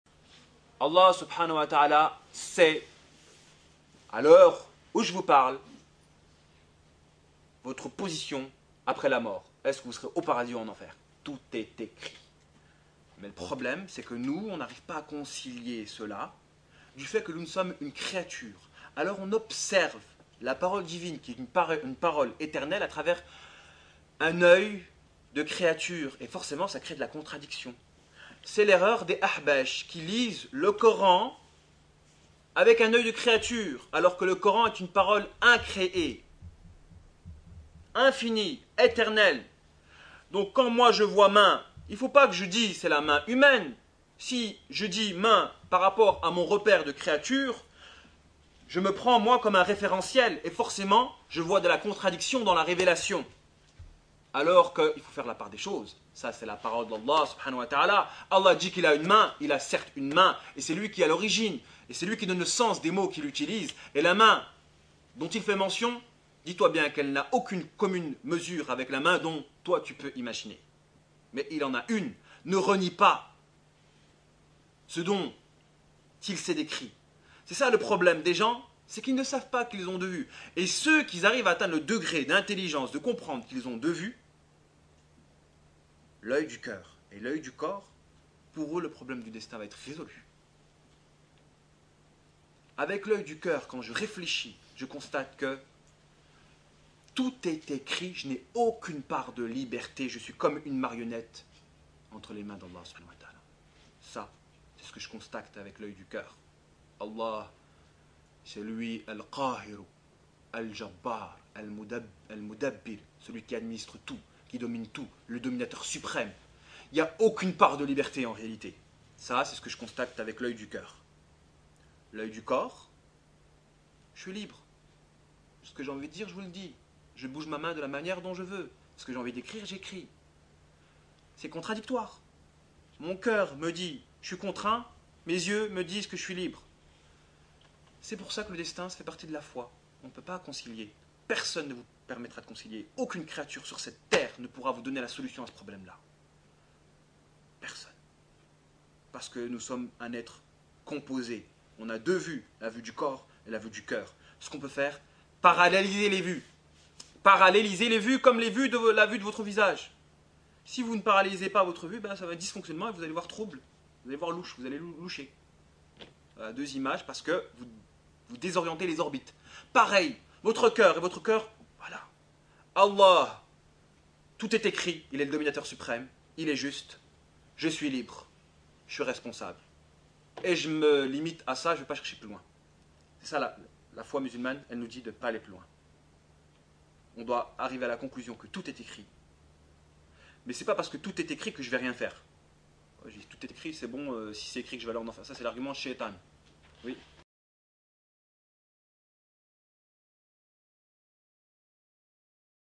élucidation sur la question du destin par Mahdy ibn Salah
une bonne vidéo qui éclaircit sur le dogme du destin et lève les contradictions